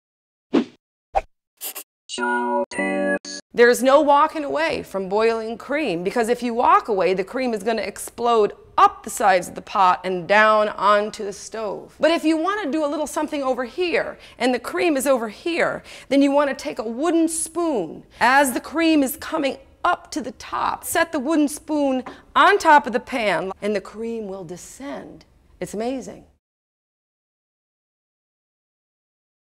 How to Stop Cream from Boiling Over - CHOW Tip
Chowhound fayehess, also known as Faye Delicious of Blip.tv, avoids a mess by placing the spoon over top of her pot.

TRANSCRIPT
There's no walking away from boiling cream because if you walk away the cream is going to explode up the sides of the pot and down onto the stove.  But if you want to do a little something over here and the cream is over here.  Then you want to take a wooden spoon, as the cream is coming up to the top, set the wooden spoon on top of the pan and the cream will descend.  It's amazing.